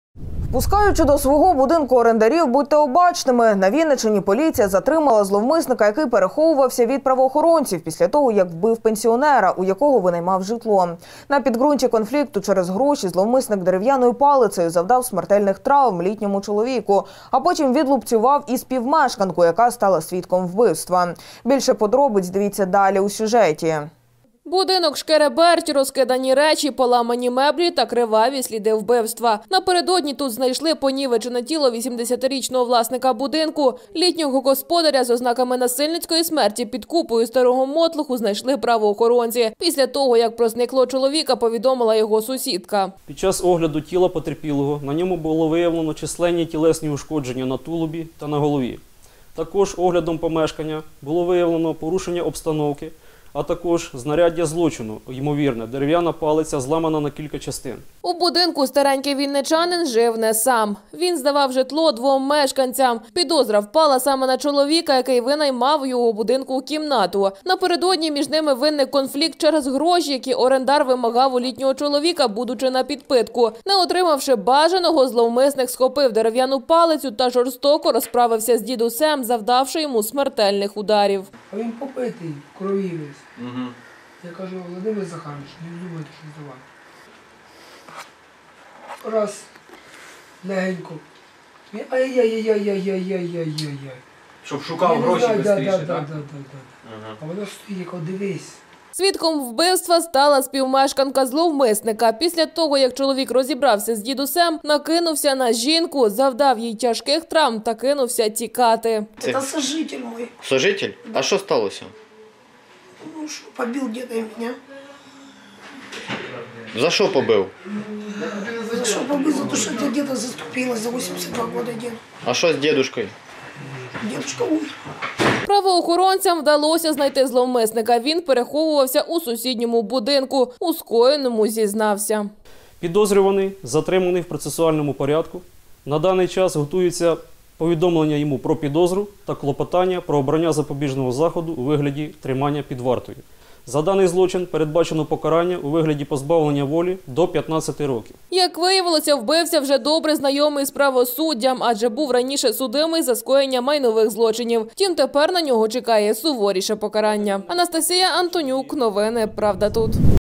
На Вінниччині поліція затримала зловмисника, який палицею вбив пенсіонера
Вбив та пограбував. На Вінниччині поліція затримала зловмисника, який переховувався від правоохоронців, після того як вбив пенсіонера, в якого винаймав житло. Напередодні між ними виник конфлікт через гроші, які орендар вимагав у літнього чоловіка, будучи напідпитку. Не отримавши бажаного, зловмисник схопив дерев’яну палицю та жорстоко розправився з дідусем, завдавши йому смертельних ударів. А потім відлупцював ще й співмешканку, яка стала свідком вбивства. Правоохоронцям вдалося знайти зловмисника, він переховувався у сусідньому будинку. У скоєному затриманий зізнався. За злочин йому загрожує до 15 років ув’язнення.